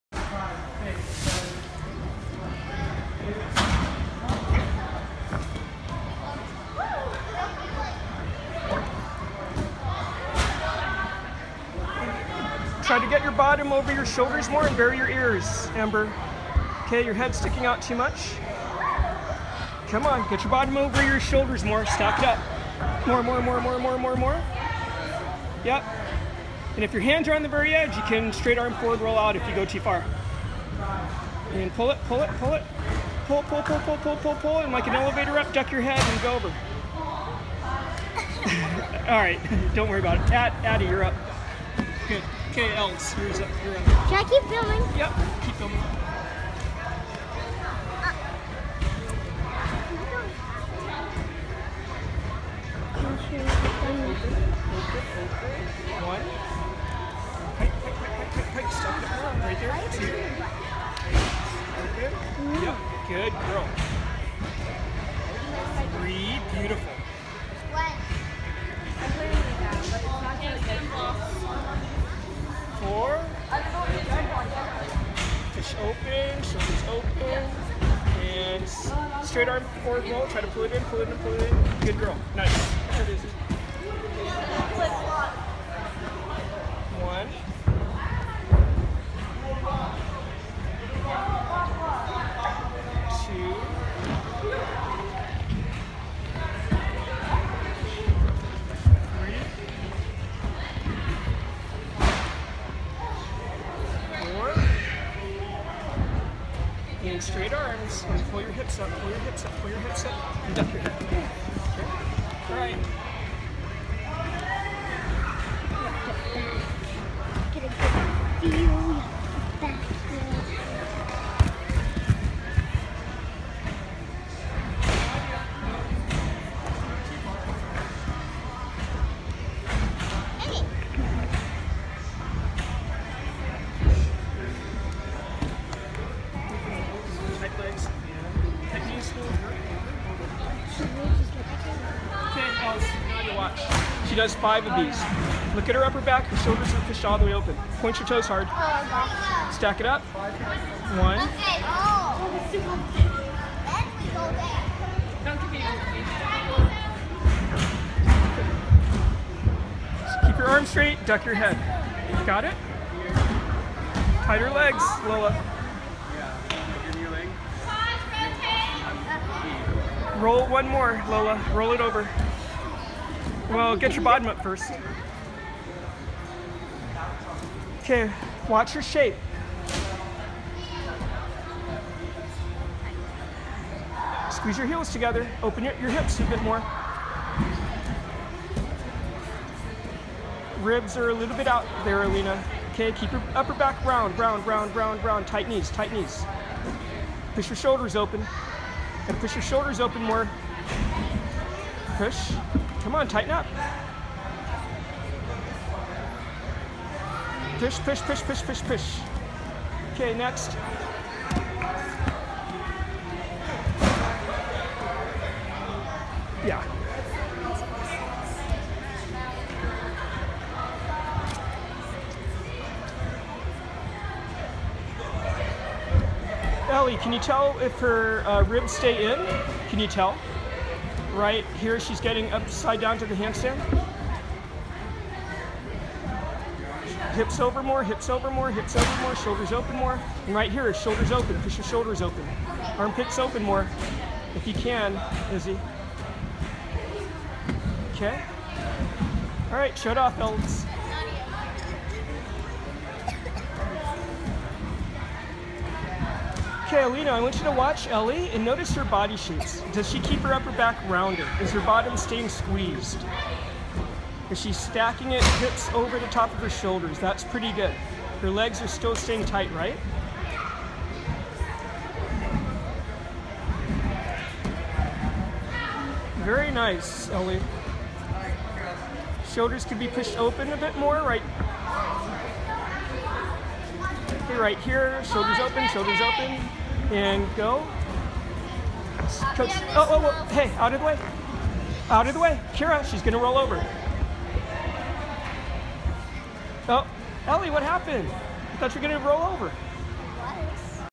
5/24/2017 AVBX6477
Beam English/cross handstand core and shoulder strengthening level 3s.